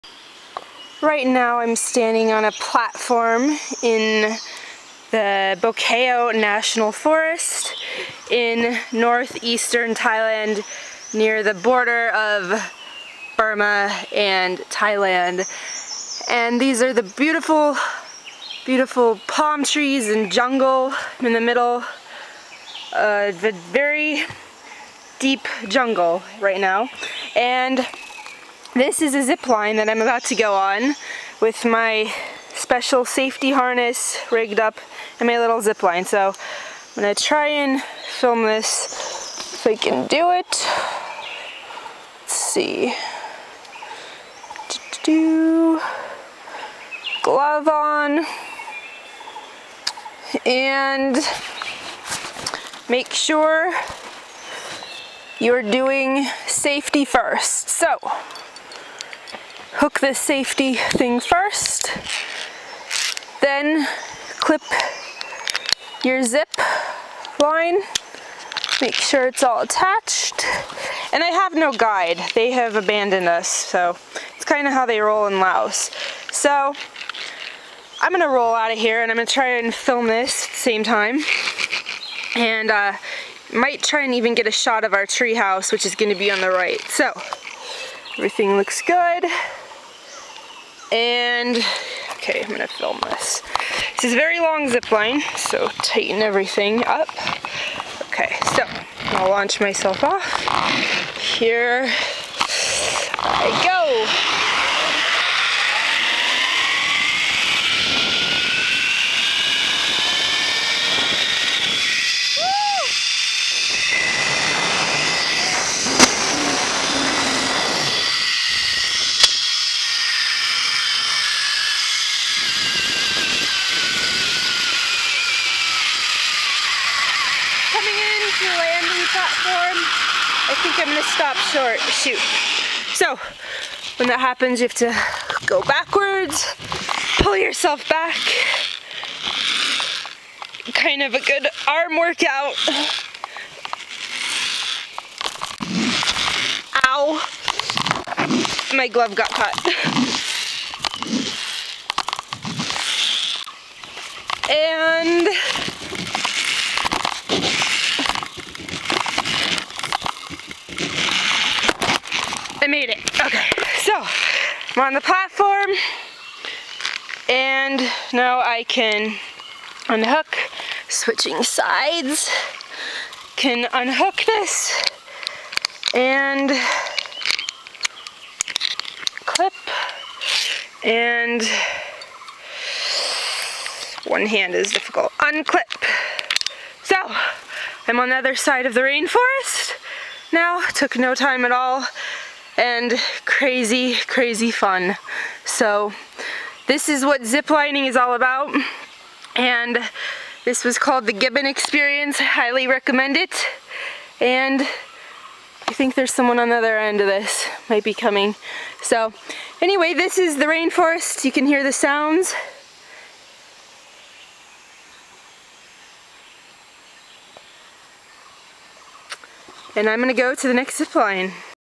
Ziplining in the Laos jungle "The Gibbon Experience" 2011
trekking and ziplining through the jungle solo. AWESOME!!! (NO guide. cause they just leave you in the jungle...alone). :P